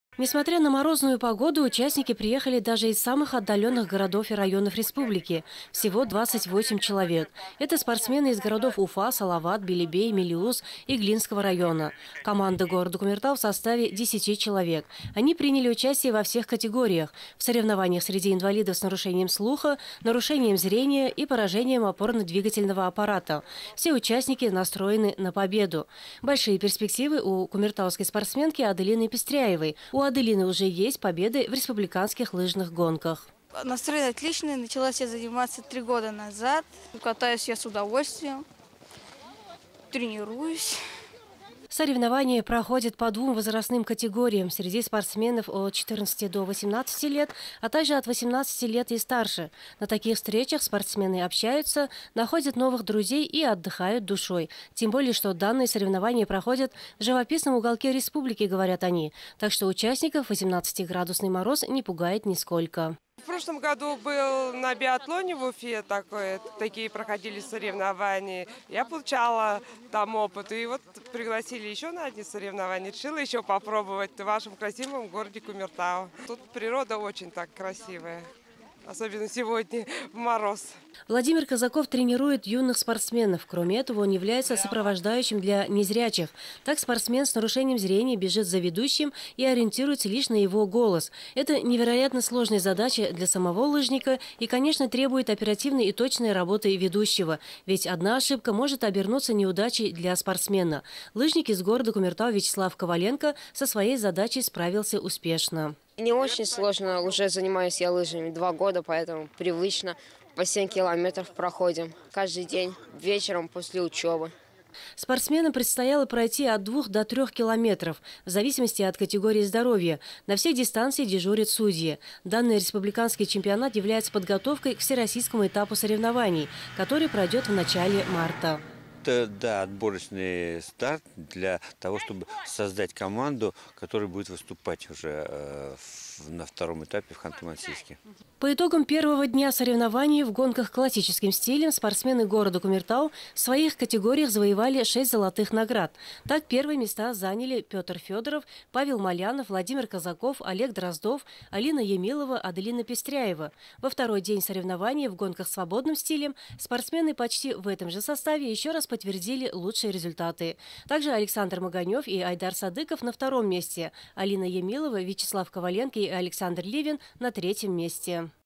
Спартакиада среди инвалидов
На минувших выходных Кумертау принял спартакиаду и чемпионат Башкортостана по лыжным гонкам среди людей с ограниченными возможностями здоровья. Команду городского округа представляли 10 спортсменов. Кому из них удалось занять призовое место, знает Айгуль Мухаметшина.